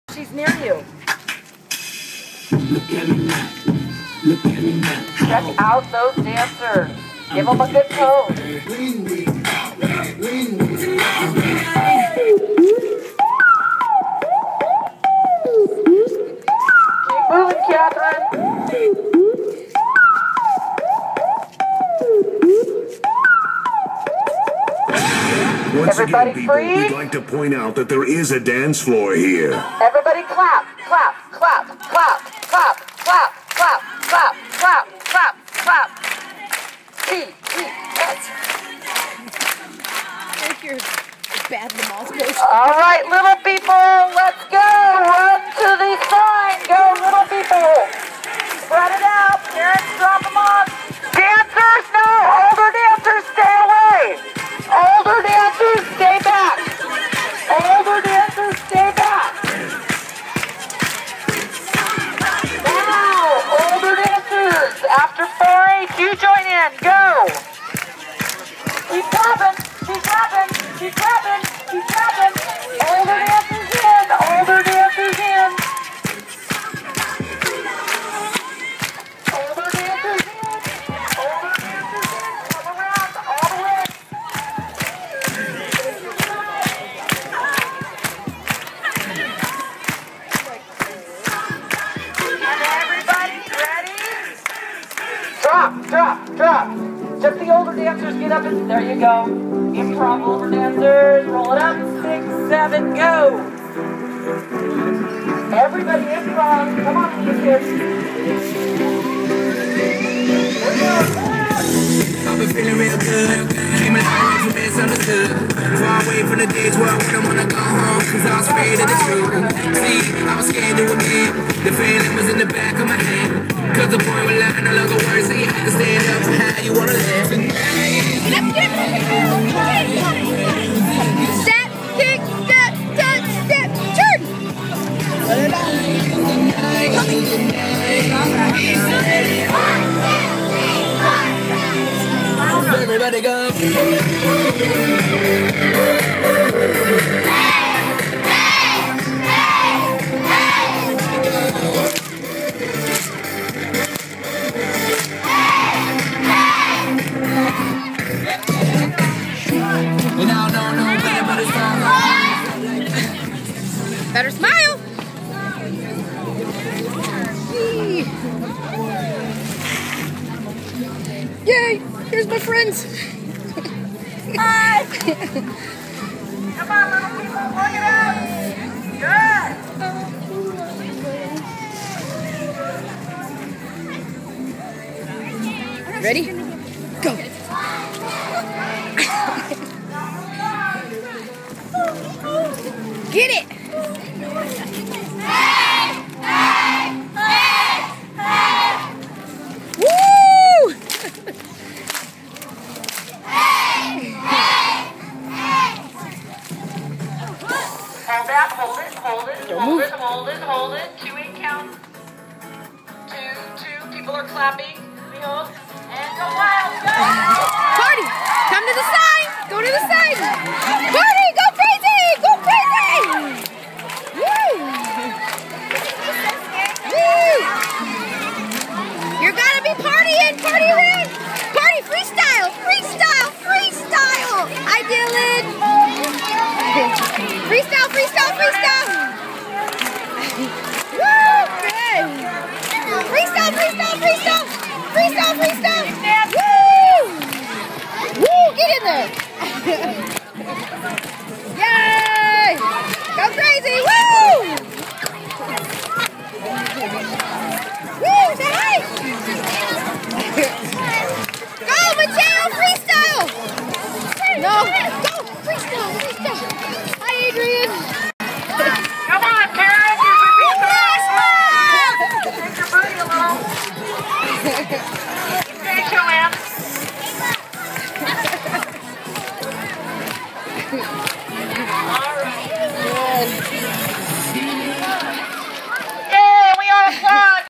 Flash mob rehearsal!
Getting ready for National Dance Week!